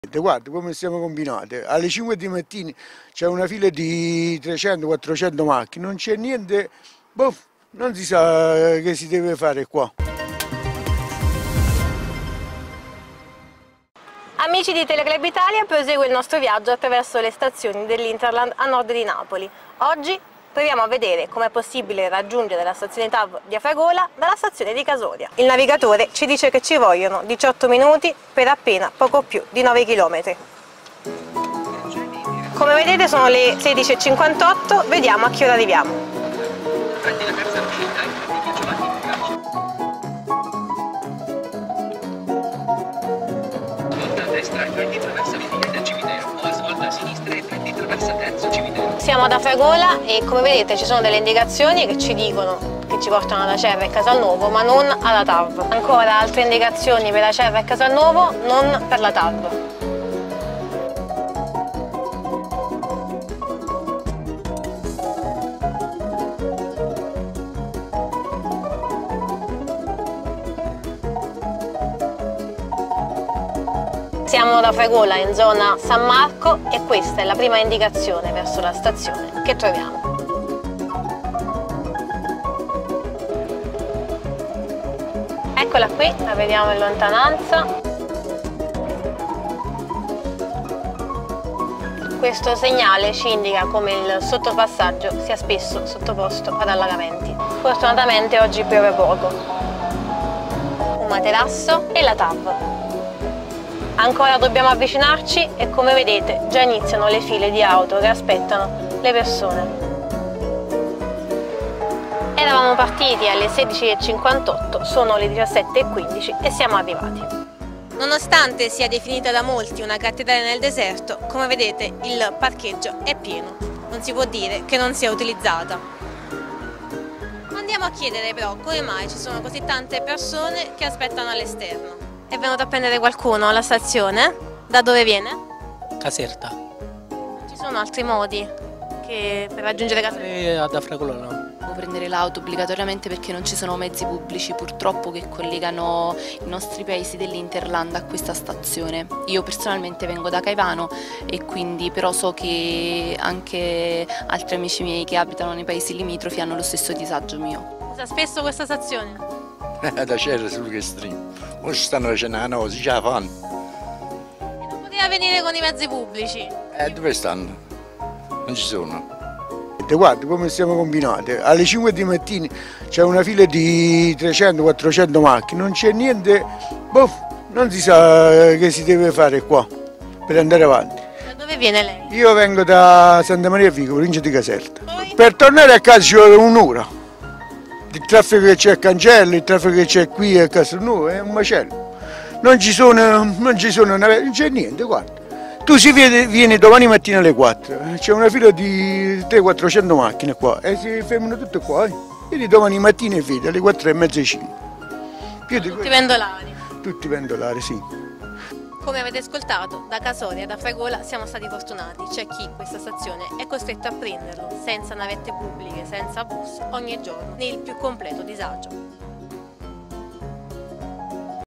Verso la TAV di Afragola: è davvero una "cattedrale nel deserto"?
Continua il nostro viaggio attraverso le stazioni dell'Hinterland. Vediamo com'è collegata quella di Casoria alla TAV della vicina